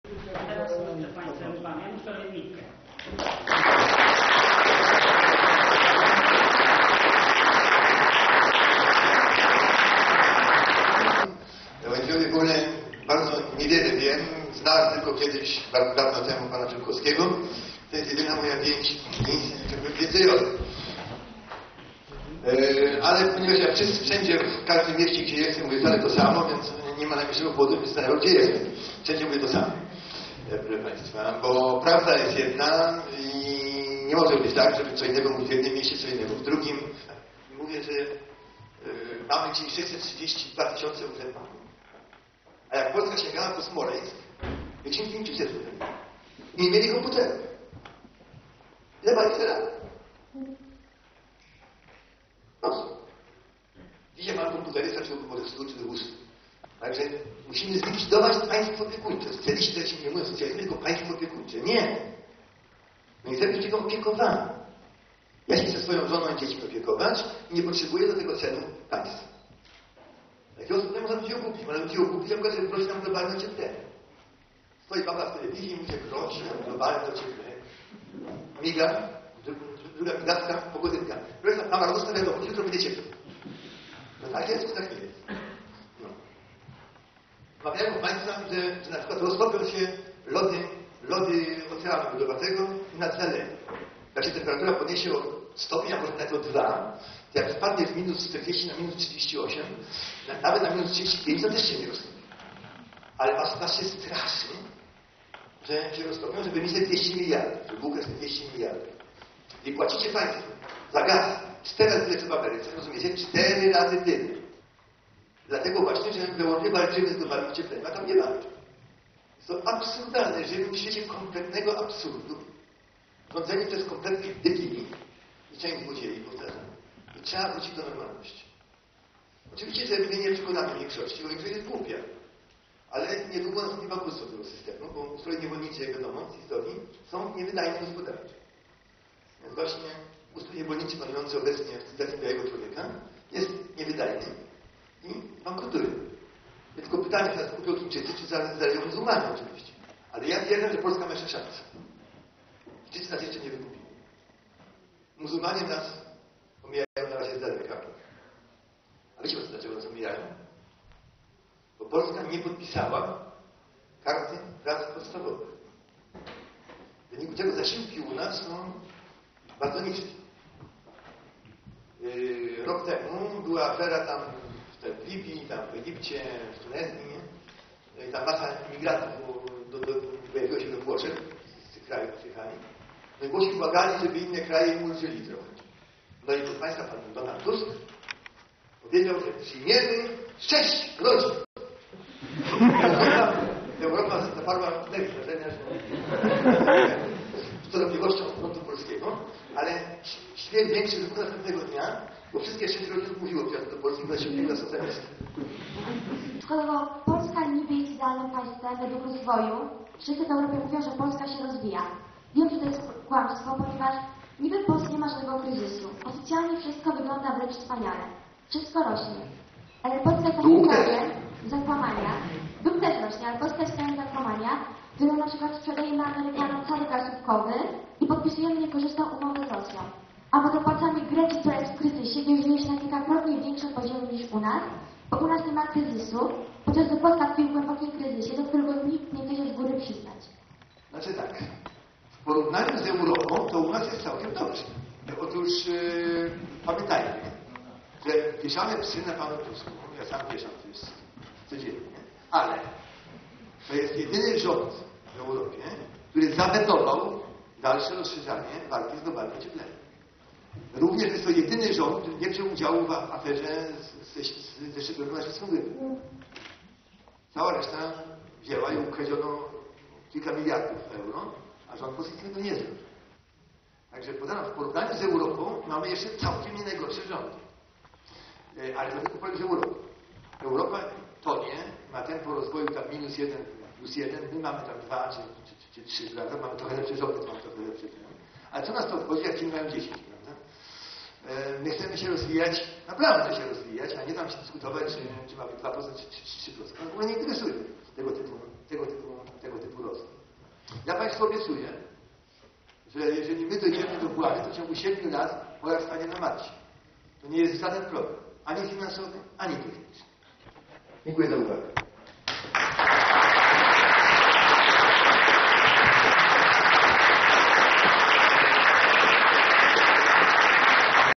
Janusz Korwin-Mikke w Jędrzejowie
Janusz Korwin-Mikke w Jędrzejowie 30 sierpnia  Centrum Kultury - relacja